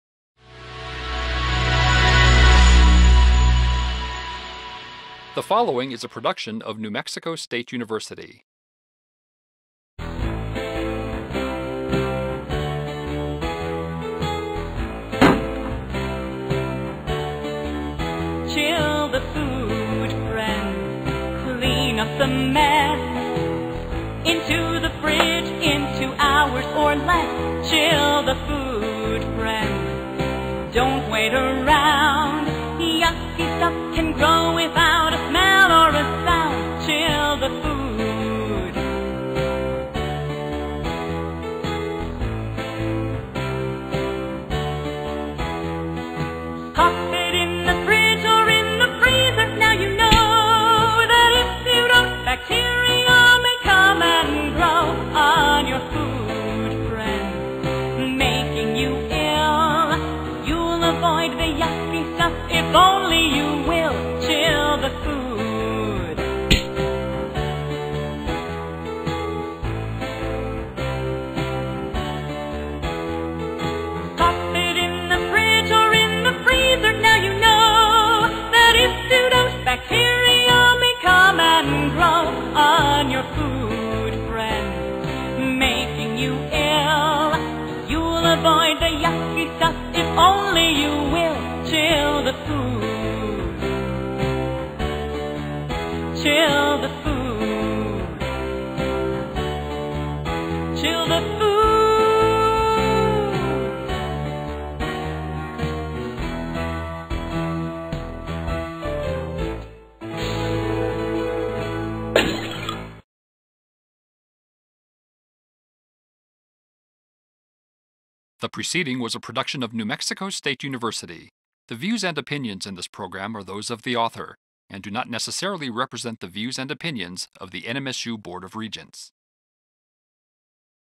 Fight BAC! Chill The Food (Spanish)
Fight BAC! Chill The Food (Spanish) - New Mexico State University 2002 -   - Video furnished by Media Productions, New Mexico State University. Noncommercial use only. 8-12 year-old kids will learn some key messages in food safety.